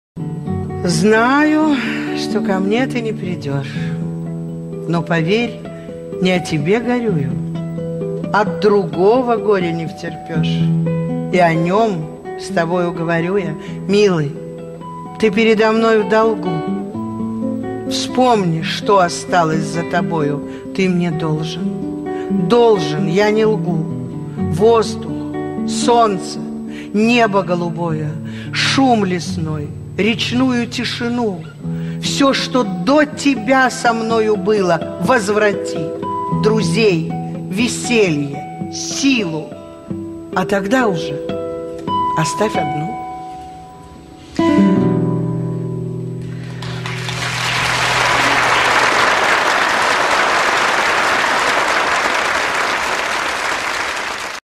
Мария Петровых. Знаю, что ко мне ты не придёшь...
Исполняет Светлана Крючкова.
Знаю, что ко мне ты не придёшь,
Но поверь, не о тебе горюю:
От другого горя невтерпёж,
И о нём с тобою говорю я...